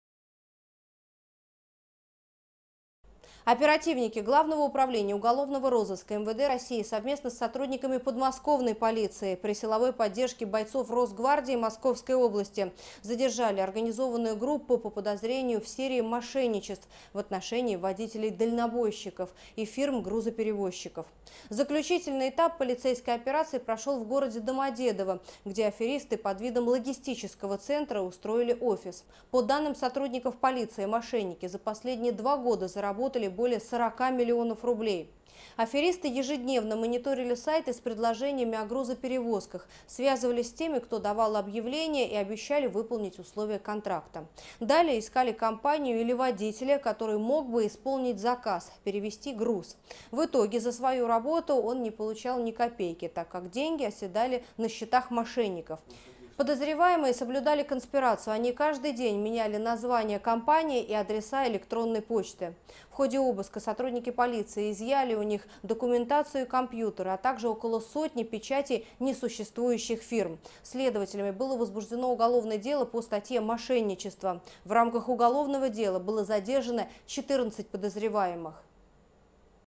Сотрудниками ГУУР МВД России задержаны подозреваемые в серии мошенничеств в сфере грузоперевозок
Оперативники ГУУР МВД России, совместно с коллегами из Подмосковья при силовой поддержке бойцов Росгвардии задержали организованную группу по подозрению в серии мошенничеств в отношении водителей-дальнобойщиков и фирм грузоперевозчиков.